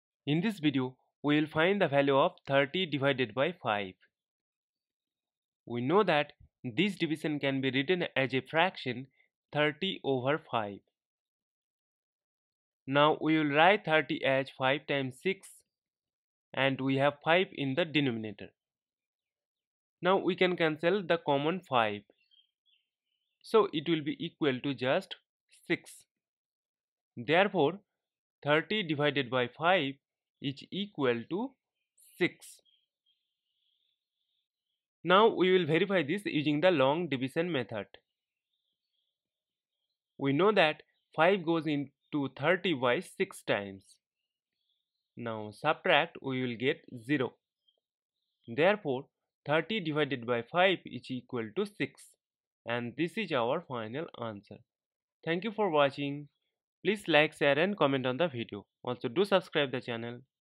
30 divided by 5
Topic: How to divide 30 by 5.

Question: Find 30 divided by 5.

Answer: 30 divided by 5 is equal to 6. That is, 30/5 =6.

We post videos daily on youtube